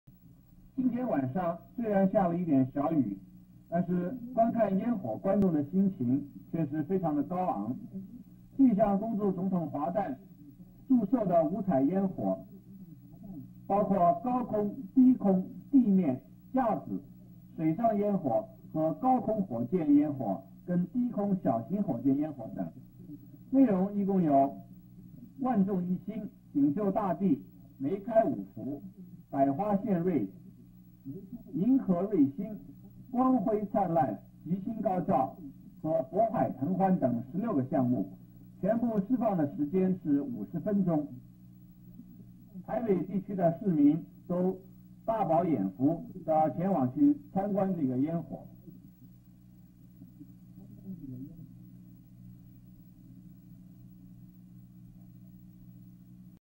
祝壽焰火
(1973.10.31)恭祝總統華誕祝壽焰火，共釋放五色煙火光輝燦爛